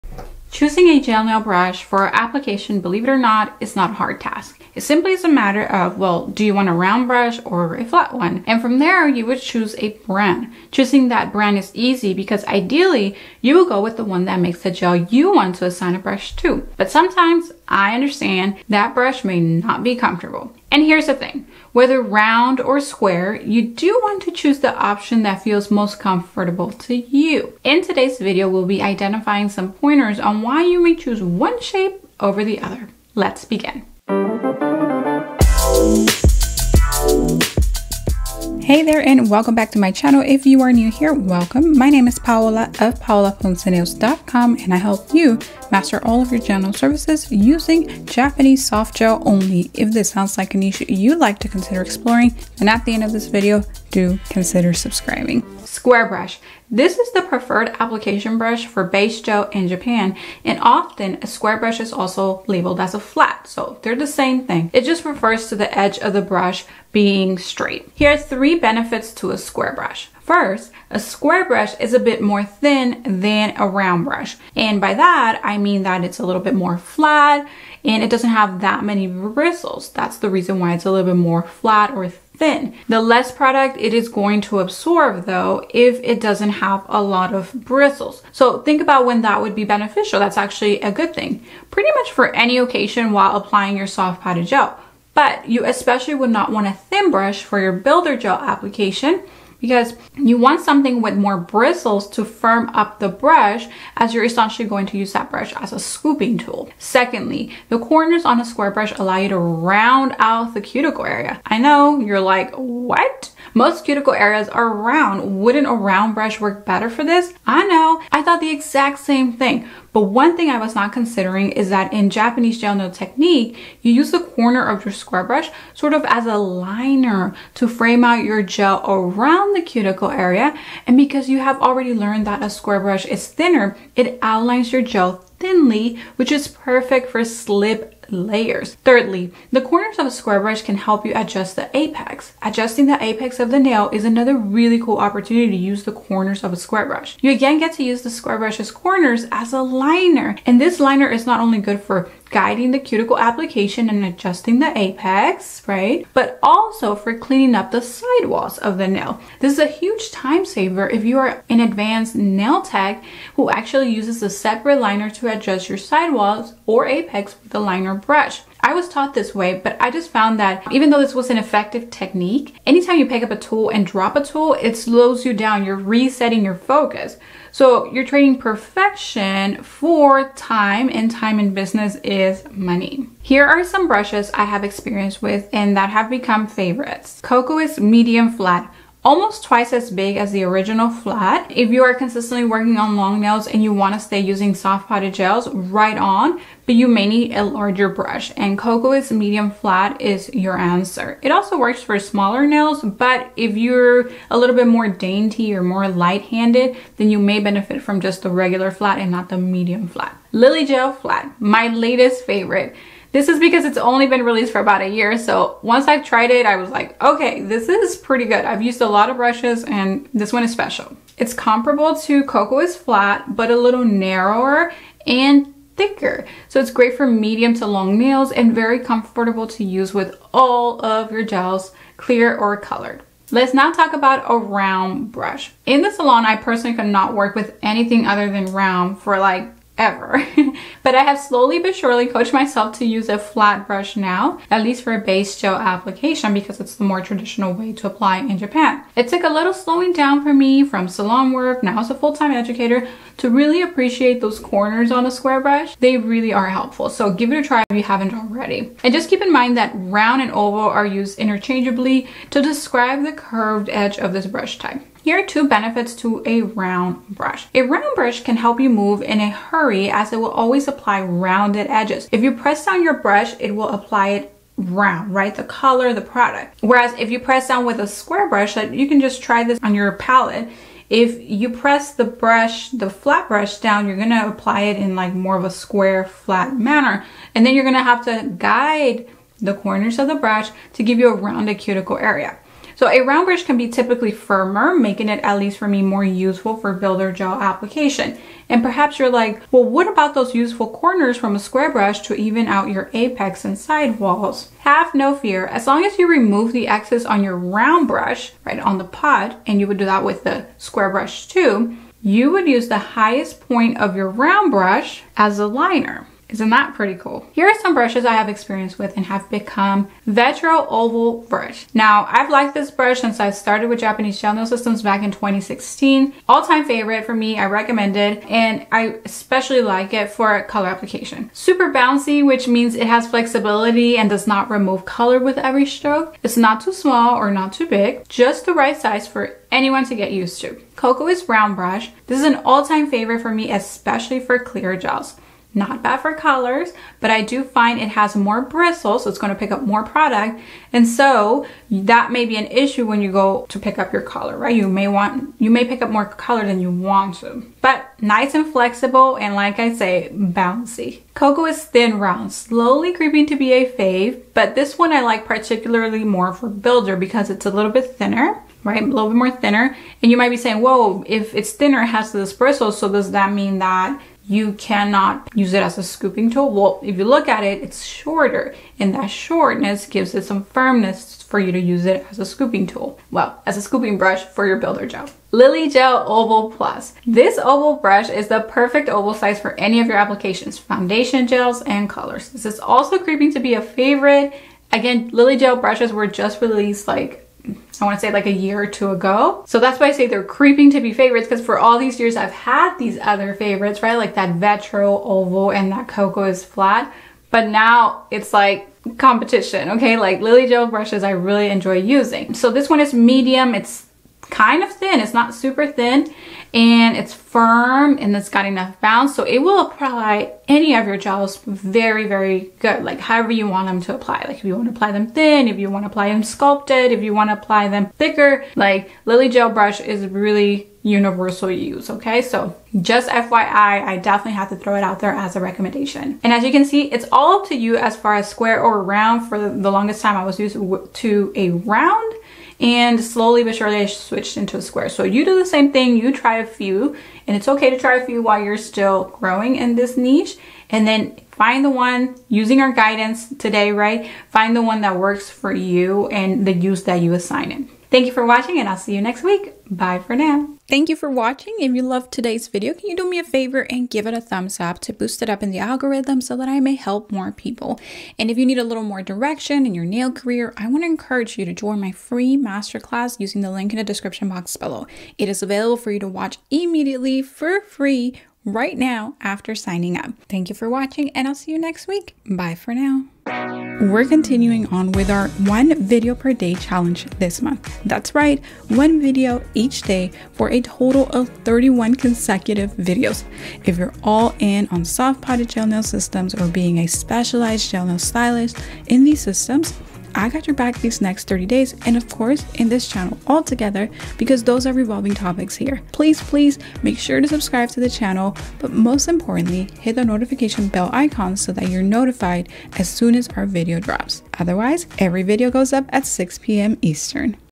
What Gel Nail Brush To Choose Square OR Round| Does it make a difference?
🔺Waitlist: MGN PRO- The Only Program you'll need to define your path as a nail tech!

🔺Today's Topic:

Choosing a gel nail brush for application, believe it or not, is not a hard task.

It simply is a matter of … well do you want a round brush, or flat one?

And from there you would choose a brand. Choosing that brand is easy because ideally you will go with the one  that makes the gel you want to assign a brush too. But sometimes, I understand, that brush may not be comfortable.

And here’s the thing, whether round or square, you do want to choose the option that feels most comfortable for you.

In today’s video we’ll be identifying some pointers on why you may choose one shape over the other.
 When you're ready hot play.